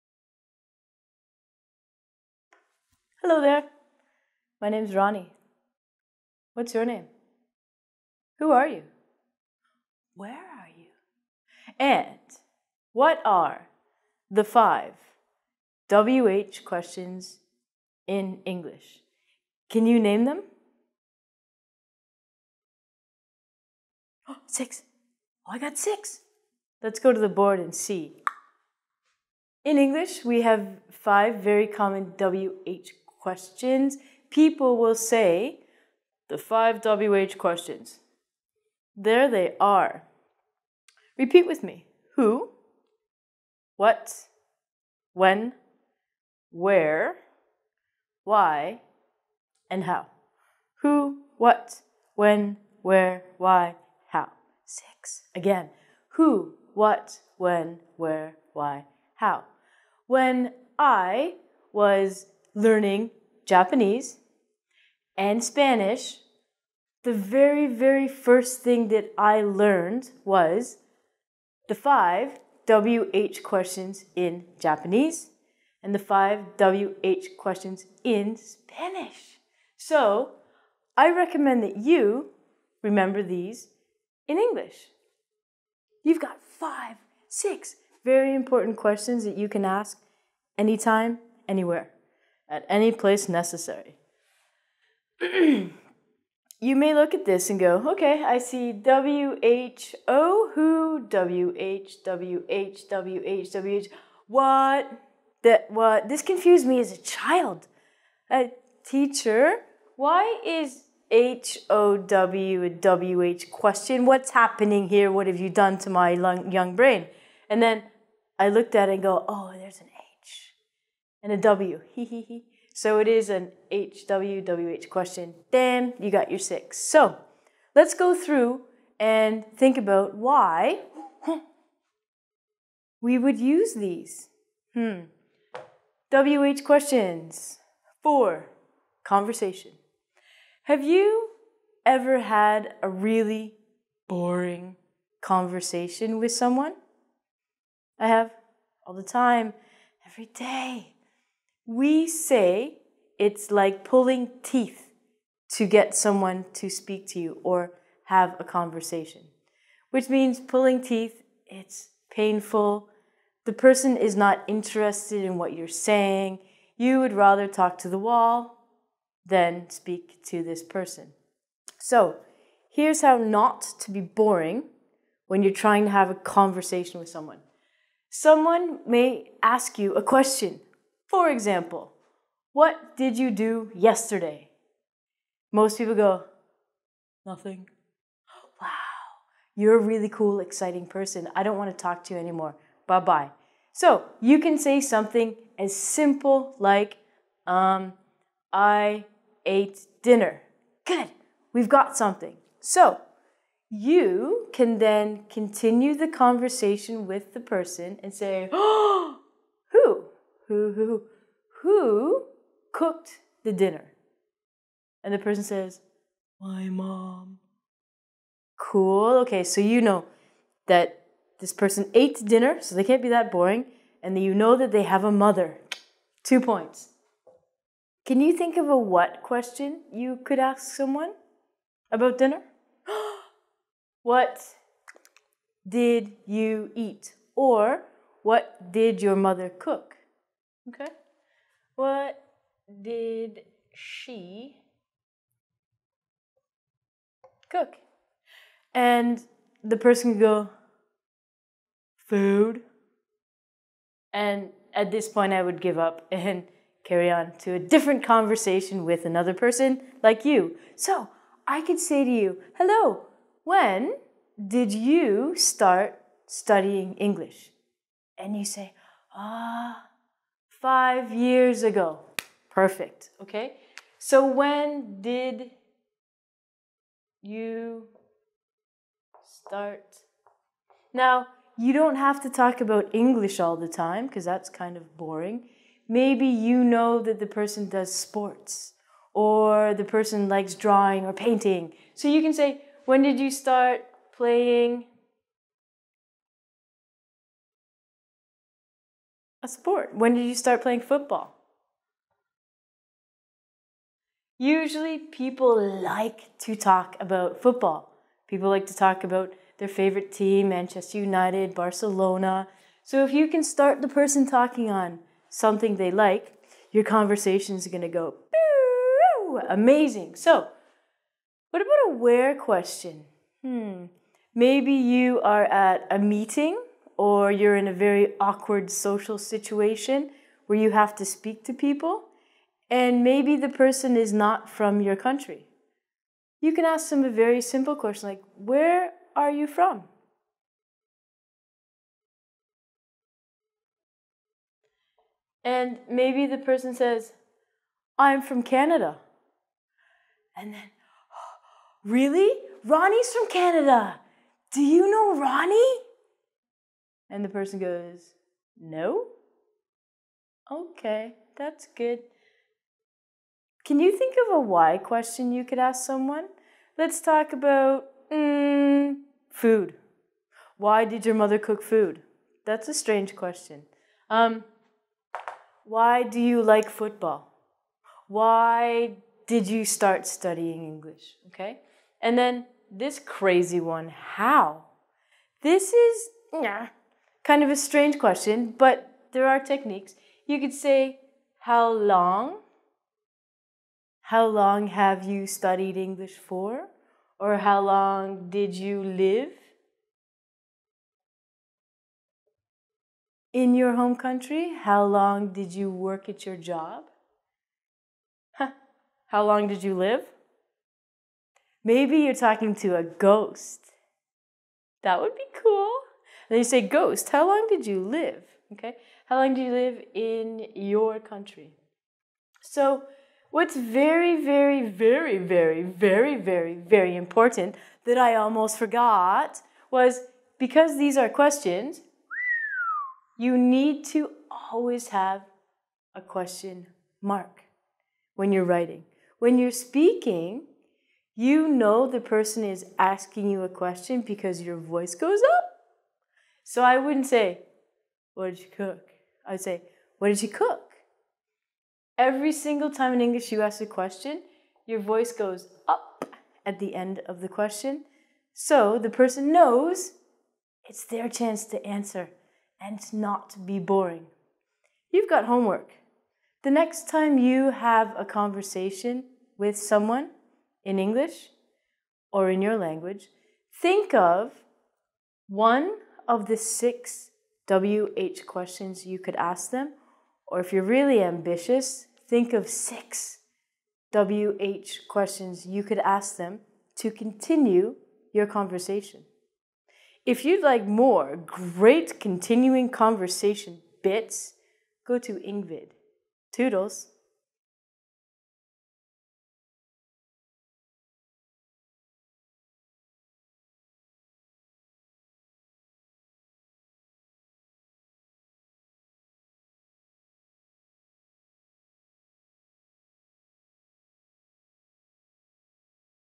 Improve your conversation skills with WH questions
Where did you go? Who did you go with? Learn how to keep a conversation going by using who, what, when, where, why, and how! Now why don't you take the quiz?